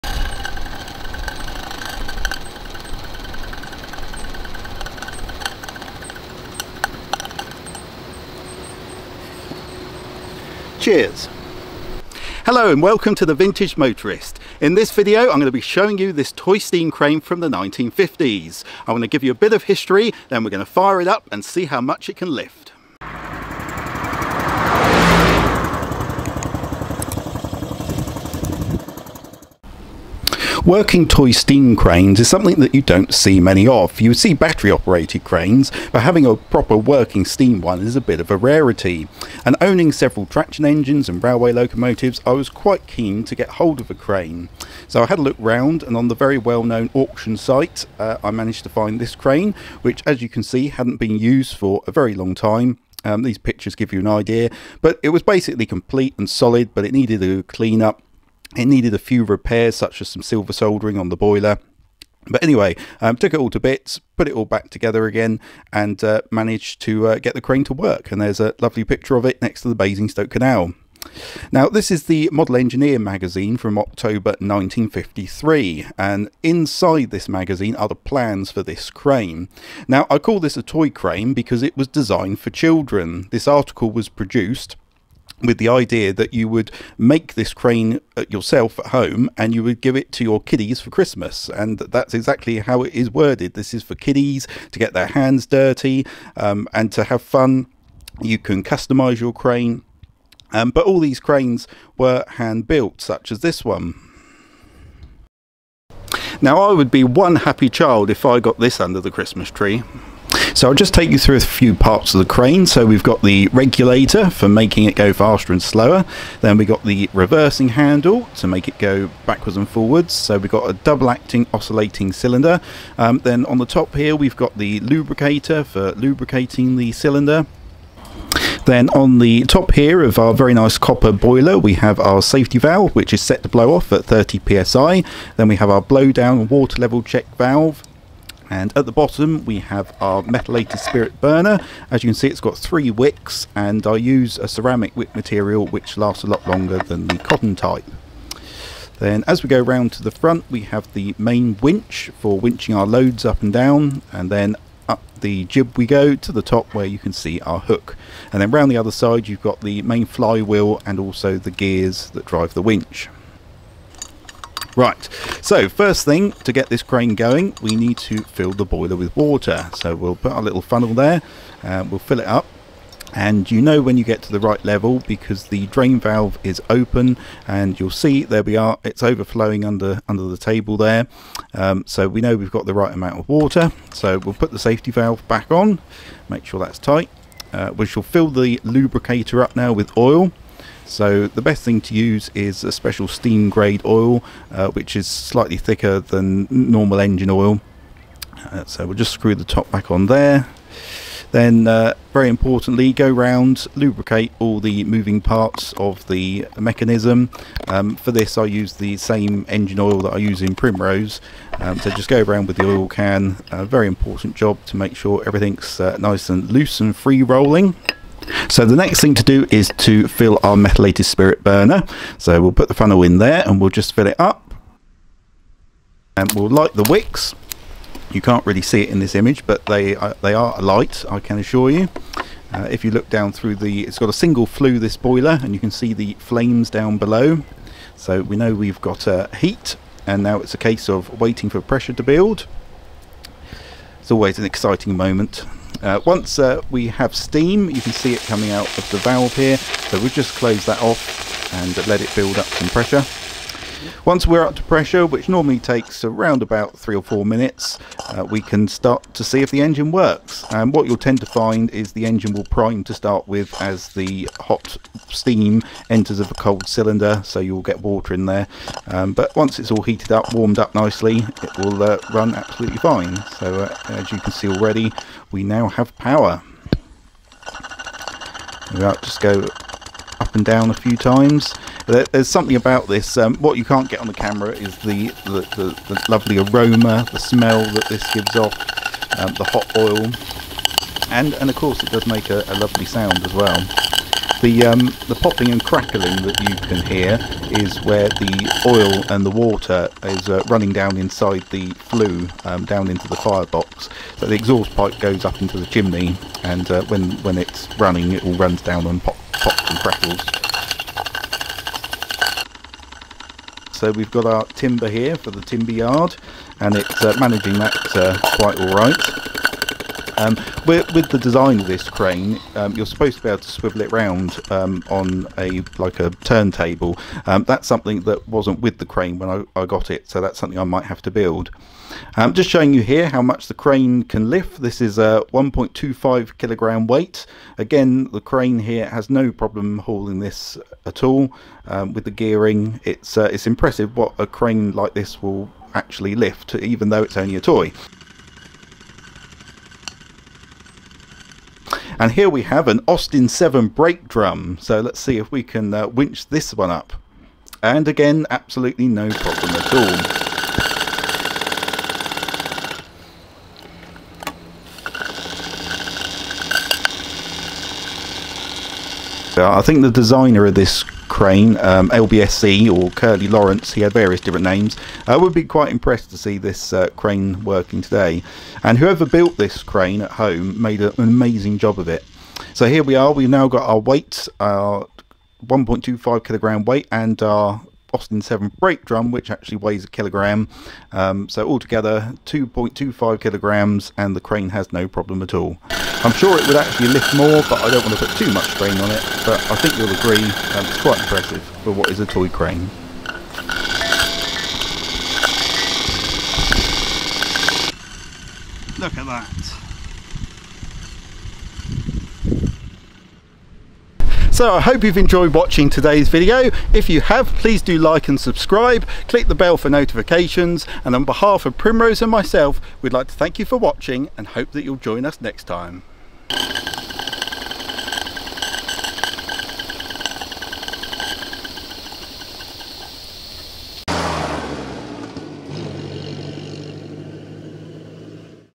Steam Crane - How it works
This lovely toy steam crane was built from the designs of Curly Lawrence published in the Model Engineer magazine of October 1953.